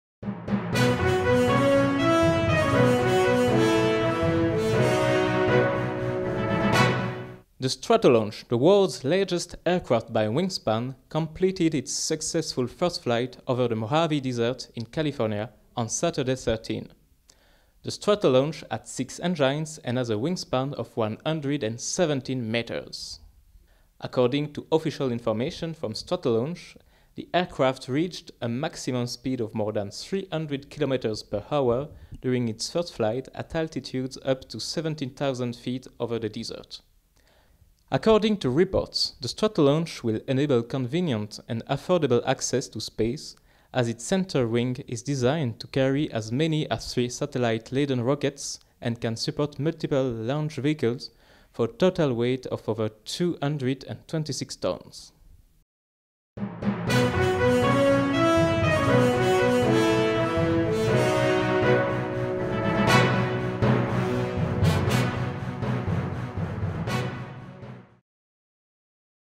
Stratolaunch largest plane in the world makes its first flight 1' Defense Breaking News
Stratolaunch Systems Corporation, founded by Paul G. Allen, successfully completed the first flight of the world’s largest all-composite aircraft, the Stratolaunch. With a dual fuselage design and wingspan greater than the length of an American football field, the Stratolaunch aircraft took flight at 0658 PDT from the Mojave Air & Space Port.